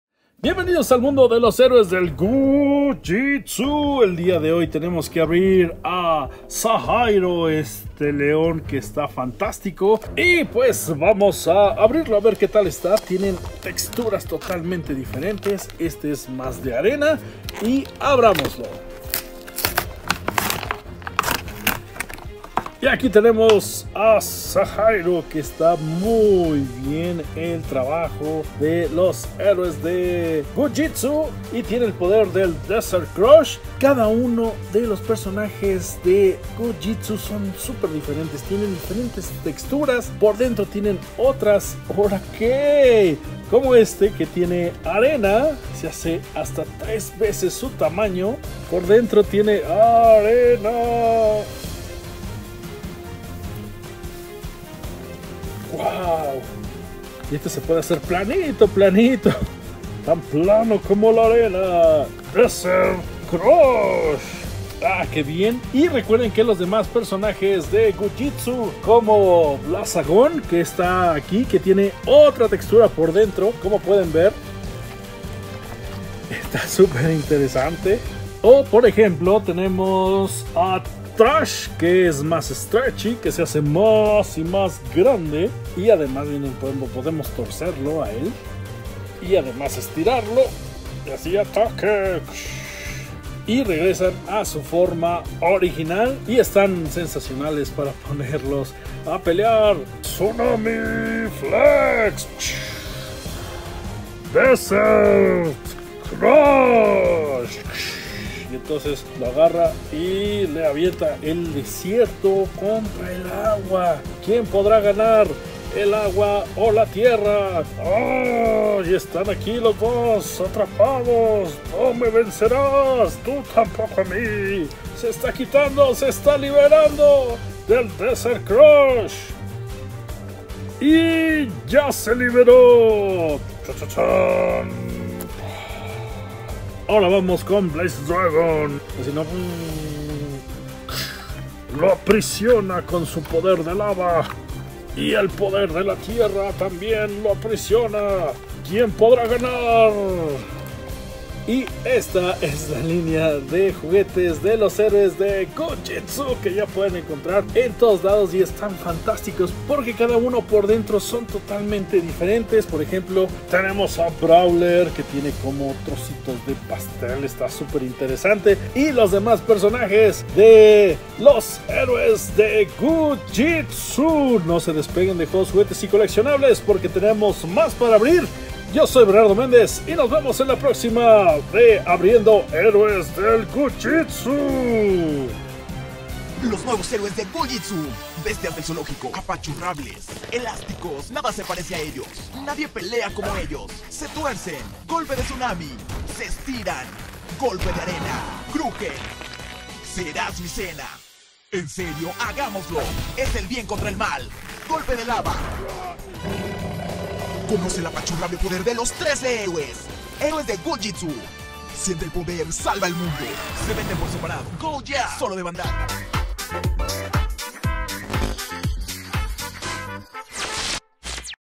🔴 Heroes de Goo Jit Zu - Llega SAHAIRO león de arena #03 goojitzu ► JJYC
Heroes de GOO JIT ZU revison de SAHAIRO Figuradeacción de león arenoso - ¡Estira, aprieta y estruja tus figuras Goo Jit Zu Heroes y mira como vuelven a su forma original!
Cada personaje tiene un relleno y textura diferente. ¡Hay 13 para coleccionar!
heroes of goo jit zu
heroesofgoojitzu
en español